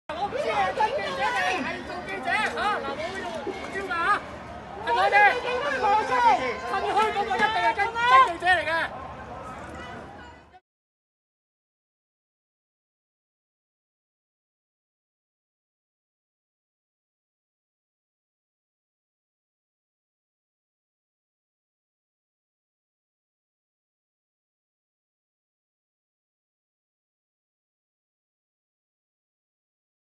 【12.24平安夜】在尖沙咀街上，警察恐嚇驅趕記者，威脅要噴胡椒水
12月24日平安夜，在尖沙咀街上，警察恐嚇驅趕記者，威脅要噴胡椒水

責任編輯：my

12月24日平安夜面世
64頁 雙語版 $30 / 電子書$20